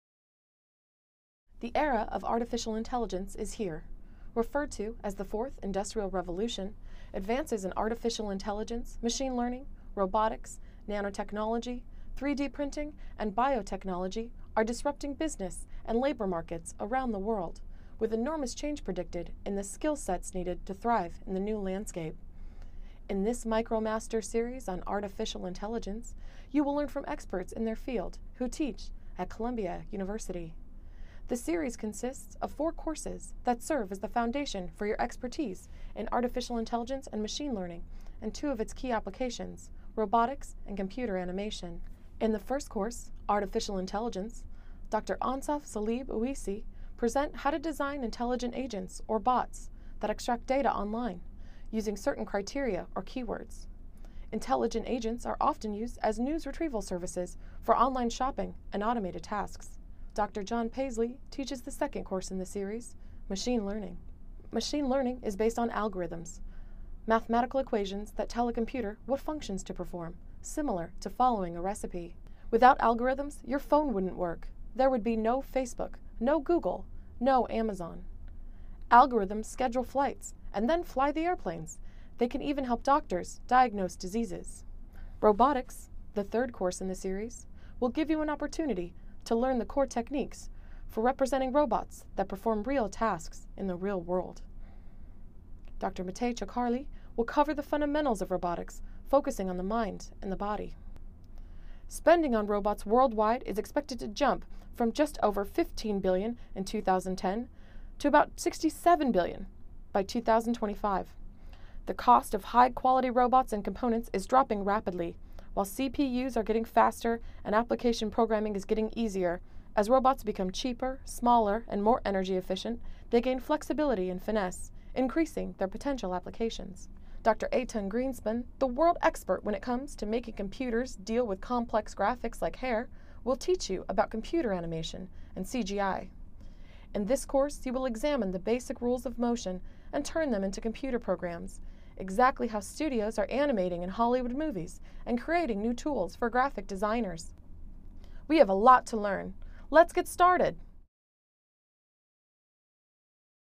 Artificial Intelligence MicroMasters® Program | ColumbiaX on edX | About Video
Gain expertise in one of the most fascinating and fastest growing areas of computer science through an innovative online program that covers fascinating and compelling topics in the field of Artificial Intelligence and its applications. This MicroMasters program from Columbia University will give you a rigorous, advanced, professional, graduate-level foundation in Artificial Intelligence.